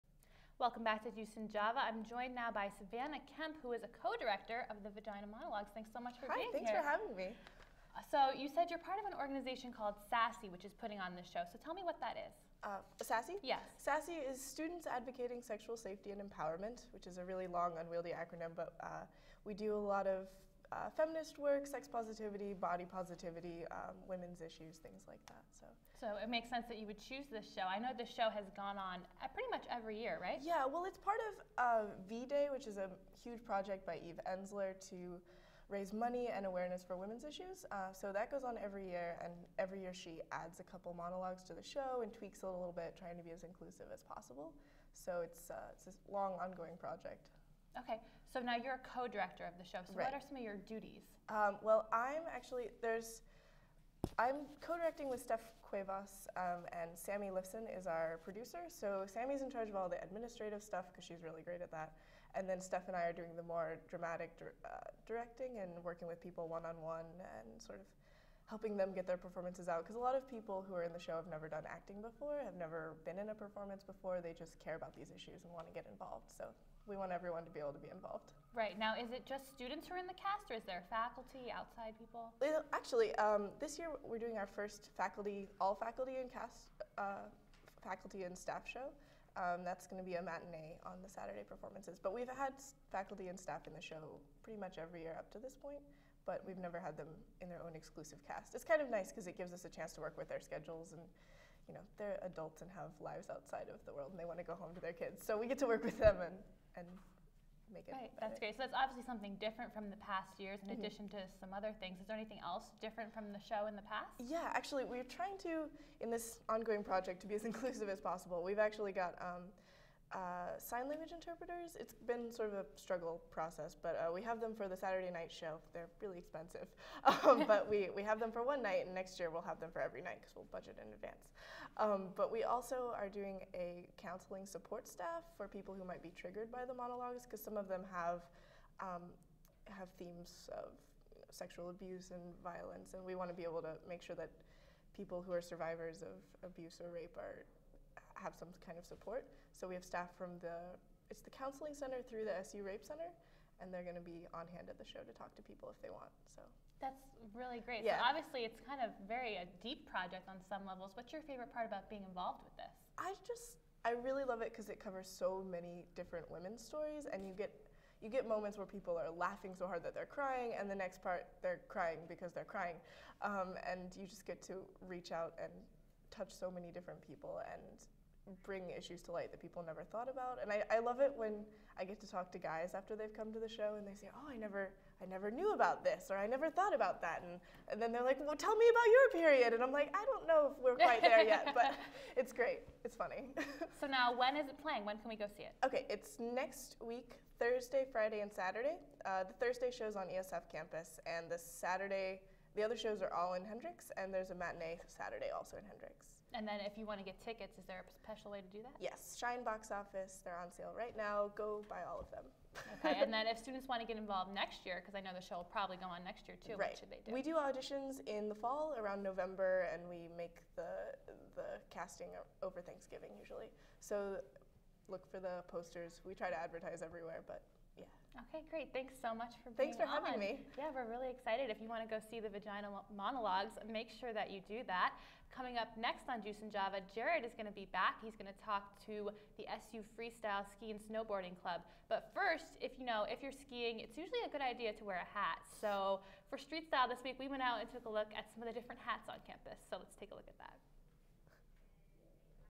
The Vagina Monologues- Juice & Java HD
Melissa Kakareka interviews Savanna Kemp, a director of The Vagina Monologues at Syracuse University